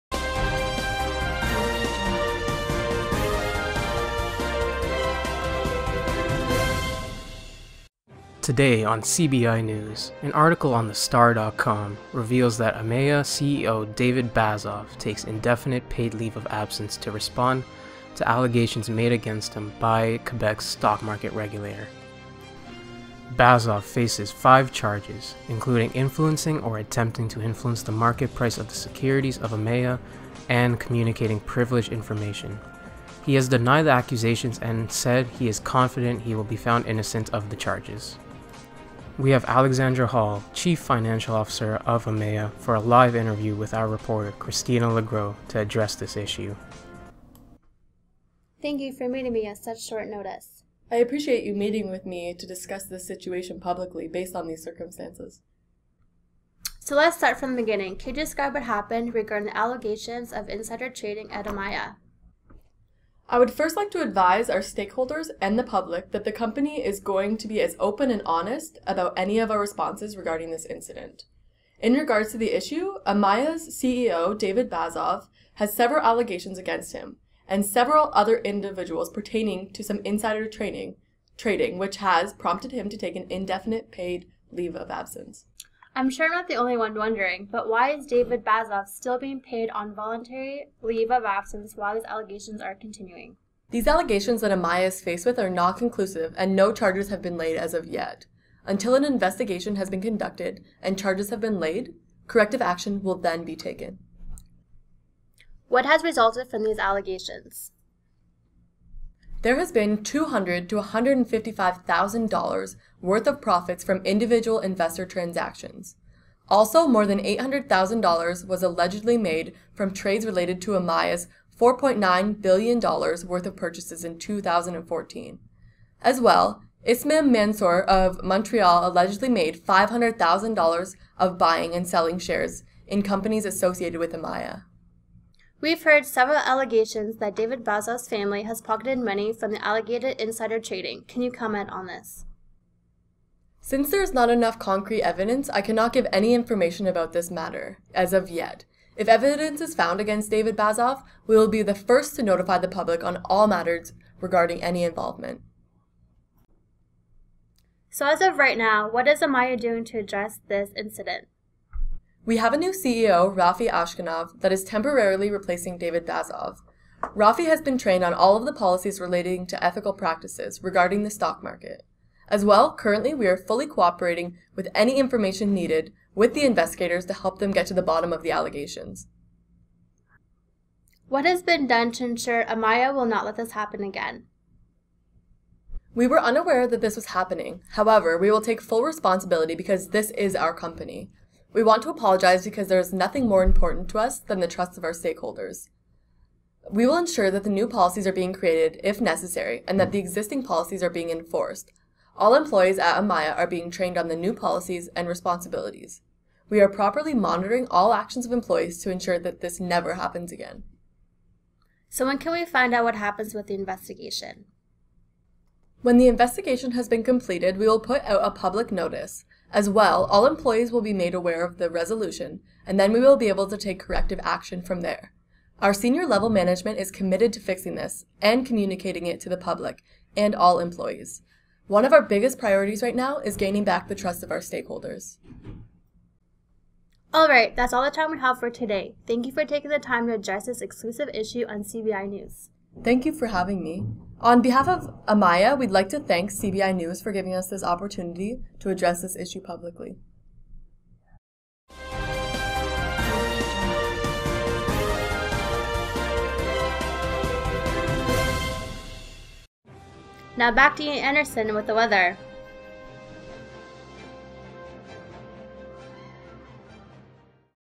MGT2325_310 Leadership Course - Team 5 - Team Crisis Assignment - Algonquin College
This is a team project where we made a video to respond to a crisis to the public on behalf of Amaya as if we were part of Amaya's crisis management team.

In this video, Christina Legrow, who plays the part of a reporter for CBI news ( A made up news station) has a one-on-one interview with Alexandra Hall, the CFO of Amaya who addresses the issue on Amaya's behalf.